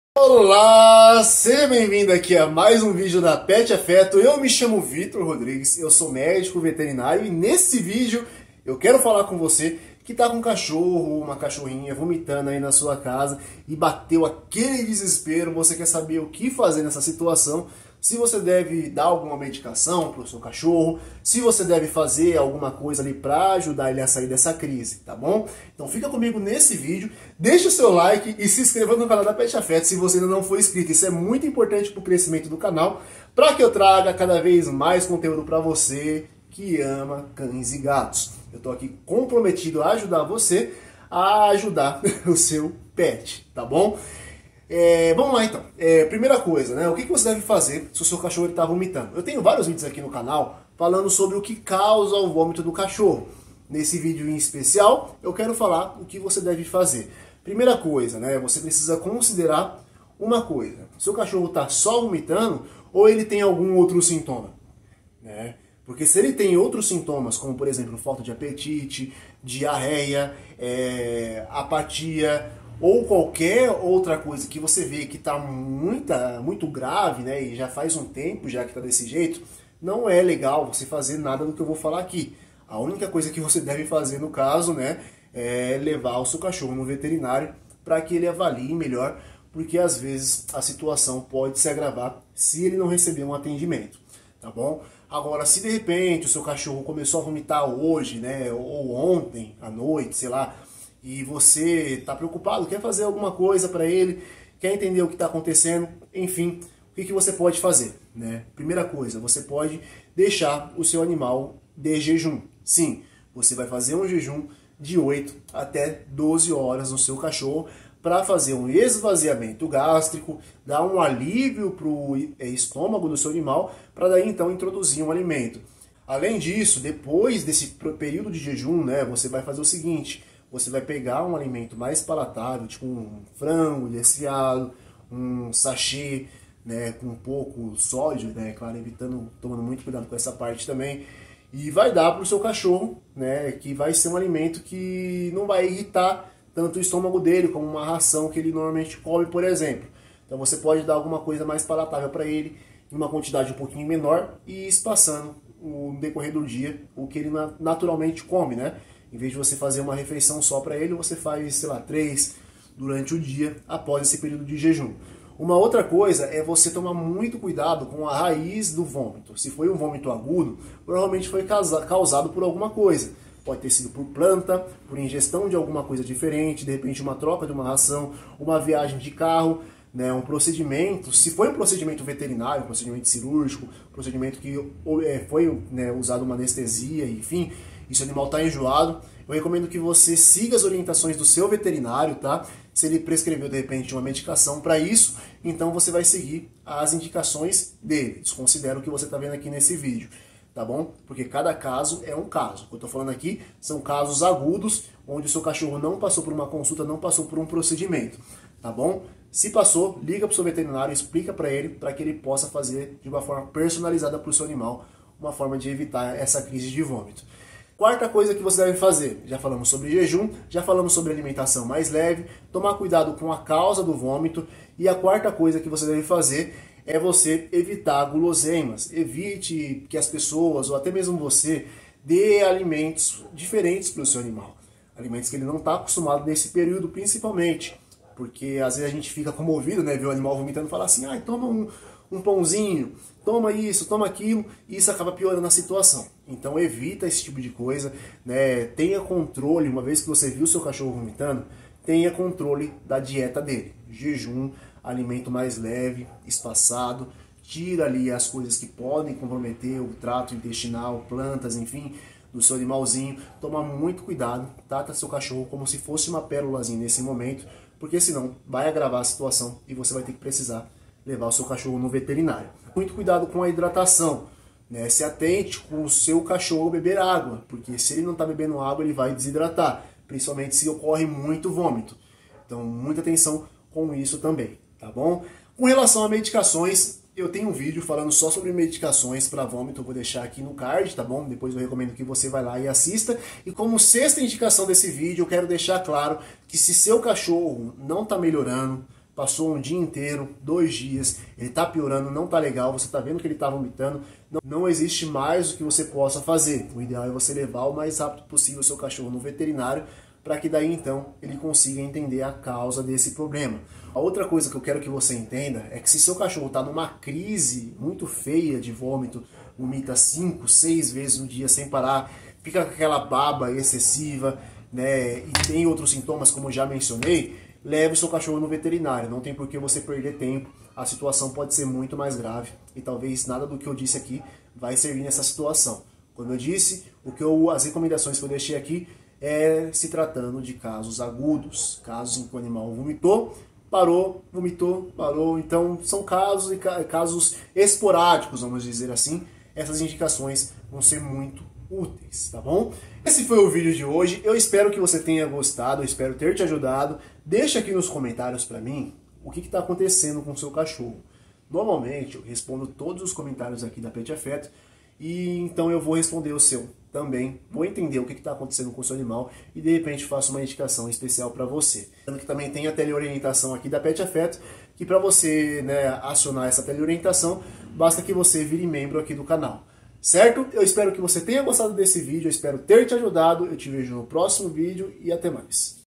O que dar para CACHORRO VOMITANDO?
Um cachorro vomitando é uma cena preocupante para os donos de animais. Isso ocorre quando o conteúdo do estômago é expelido pela boca, acompanhado de sons de engasgo. Pode ser de várias cores e consistências. Pode indicar ingestão de algo impróprio, alergias, mudanças na dieta ou problemas mais graves. Observar o comportamento pós-vômito é crucial. Vômitos frequentes, com sangue ou sintomas adicionais, exigem atenção veterinária. O diagnóstico e tratamento dependem da causa subjacente.